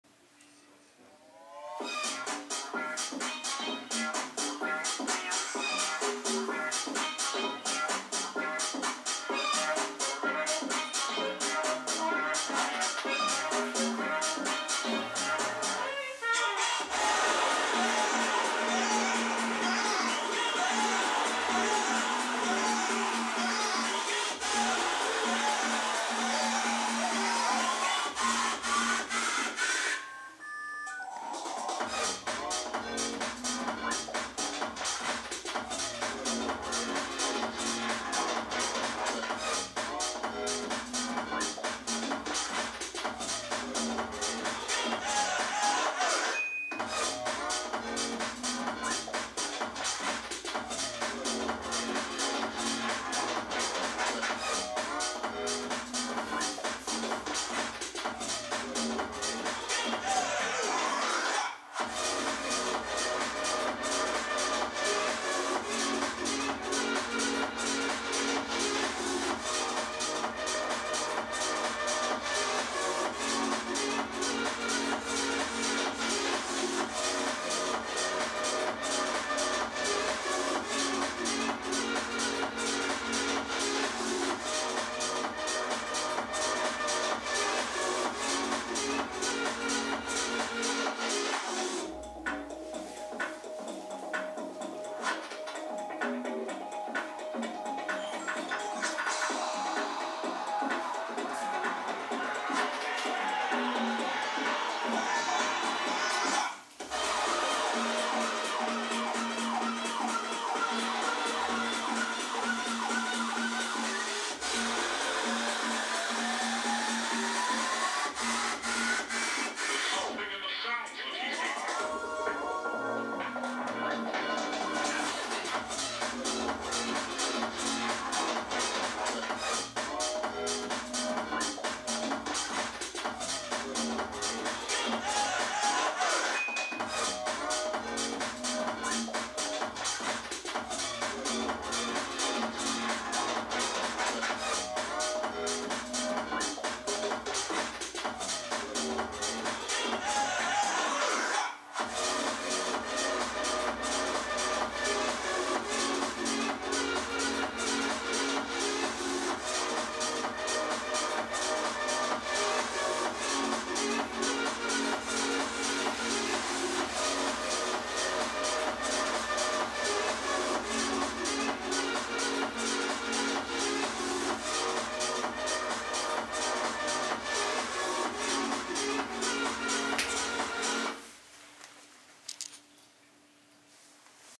SPOTIFY POSTER FULL SONG
How it works:
The device has 16 million color capacity. While the device changes the color of the light according to the dominant frequencies in the sound, it also controls its brightness according to the instantaneous level of the sound.